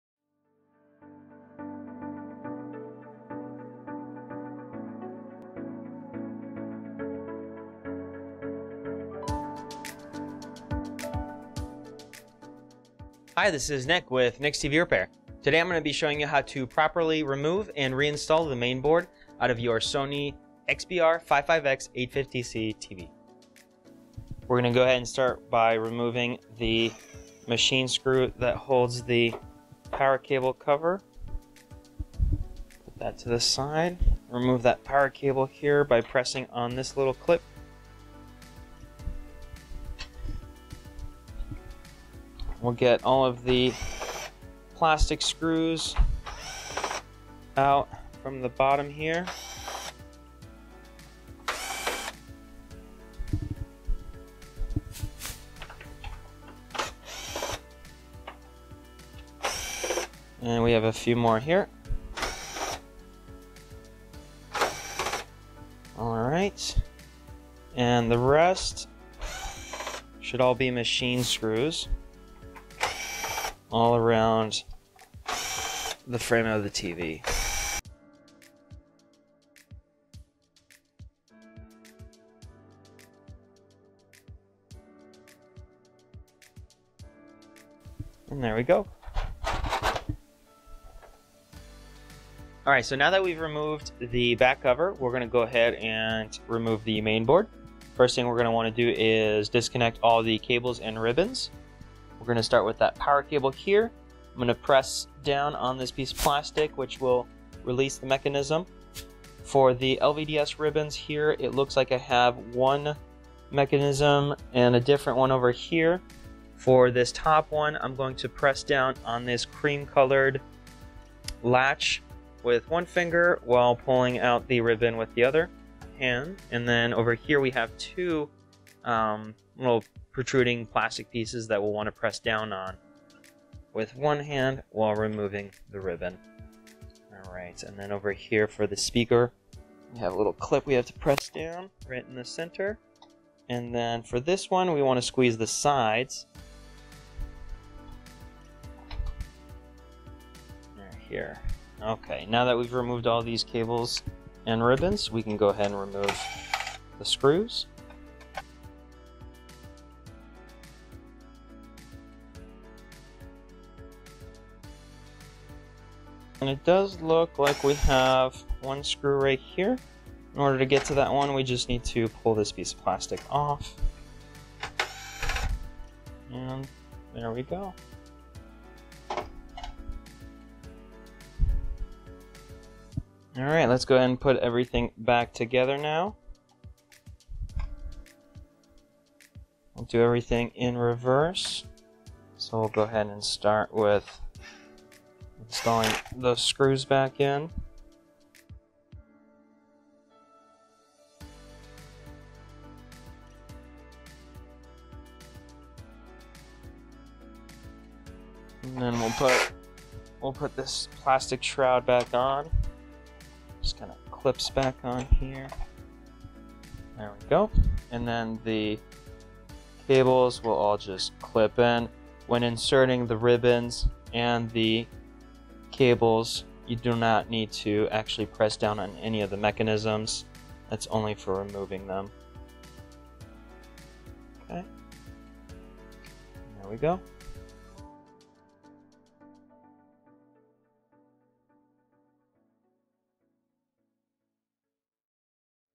Sony 4K XBR-55X850C disassembly Removal of Main board
Sony XBR-55X850C mainboard removal and re-installation process.

If you are local, feel free to pass by for help with your devices!
Nick's TV Repair,
5445 E 22nd St

Soldering Iron

Desolder wick